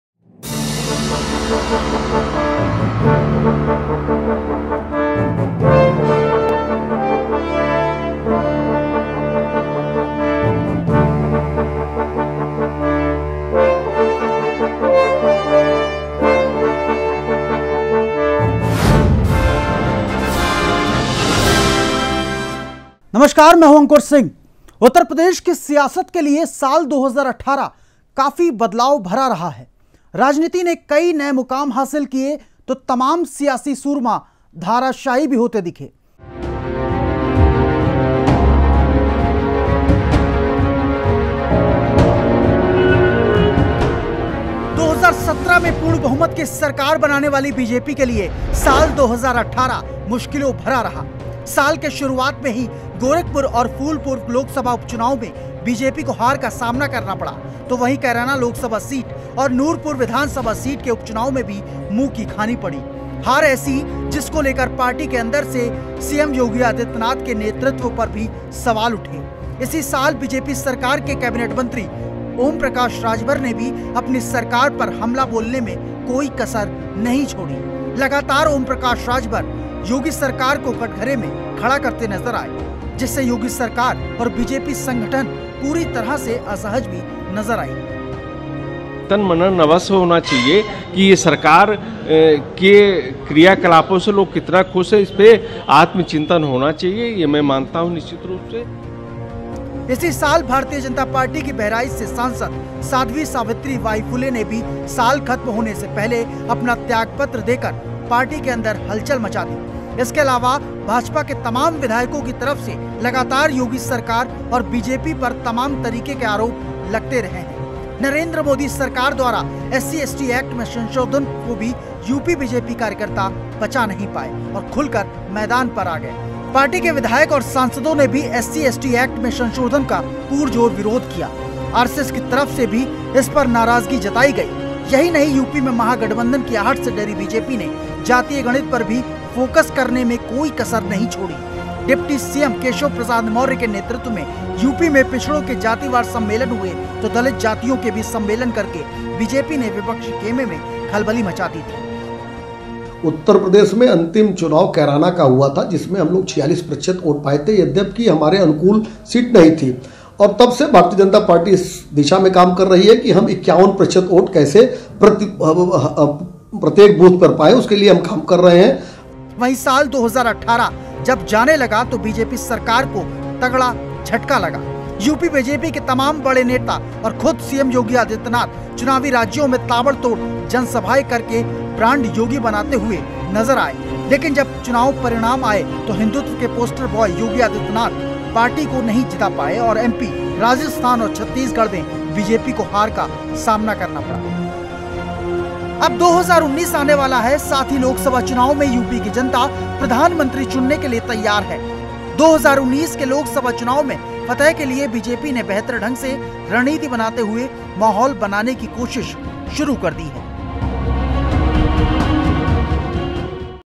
YEAR ENDER: यूपी बीजेपी के लिए विवादों से भरा रहा साल 2018
लखनऊ: उत्तर प्रदेश की सियासत के लिये साल 2018 काफी बदलाव भरा रहा. राजनीति ने कई नये मुकाम हासिल किये तो तमाम सियासी सूरमा धाराशायी भी होते दिखे. लेकिन वनवास खत्म कर वापस सत्ता में आई भाजपा के लिए ये साल काफी मुश्किलों भरा रहा.